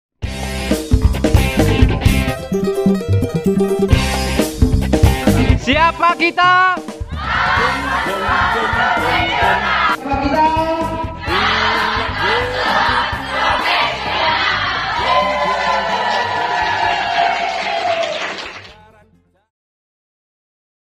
CALON KONSELOR Muda
Ucapan adalah doa, DOa adalah Harapan, Harapan Untuk Hari esok. kita kan jadi konselor profesional yang bekerja karena Tuhan Yang Maha Esa.
video di dalam adalam adalah kompilasi ucapan siapa kita. !? CALON KONSELOR PROFESIONALA Sebagian Mahasiswa IAIN Kudus Jurusan Bimbingan Konseling Pendidikan Islam.
Lokasi pengambilan
Instagram HMPS BKPI IANI KUDUS